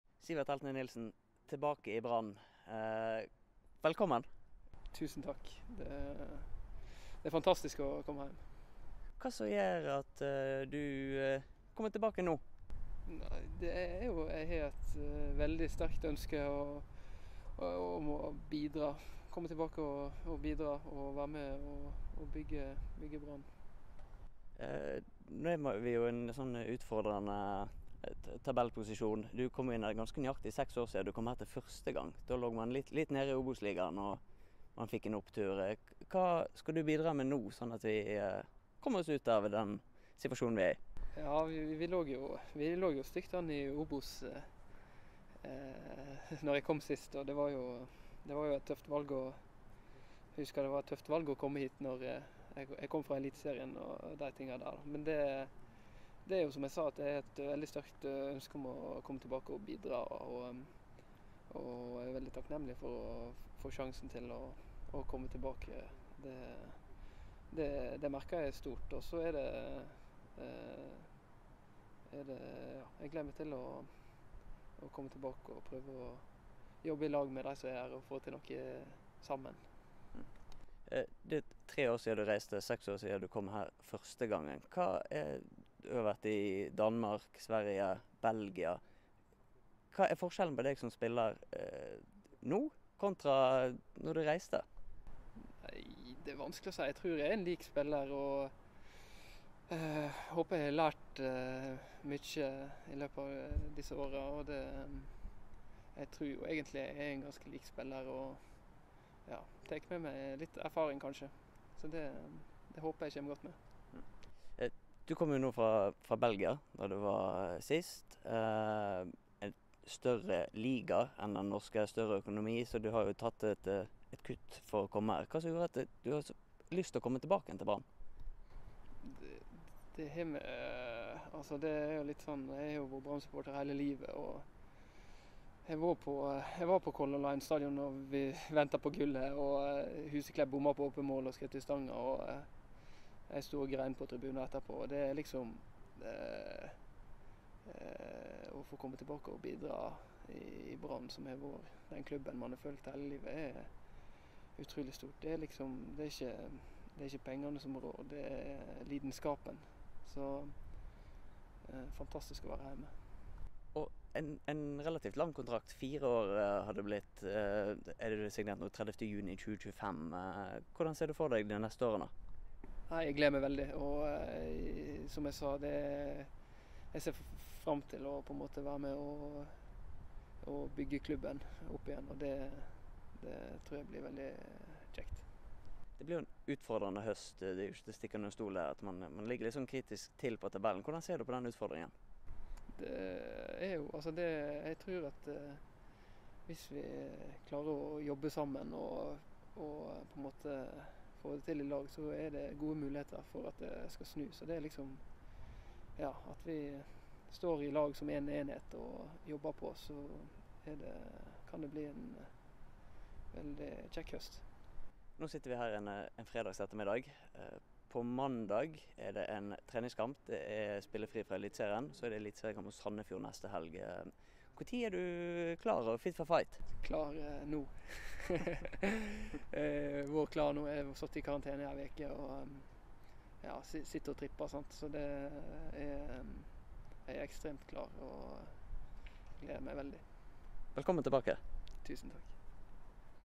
–Det er fantastisk å komme hjem
Sivert Heltne Nilsen er tilbake i Brann og har signert en avtale til 30. juni, 2025.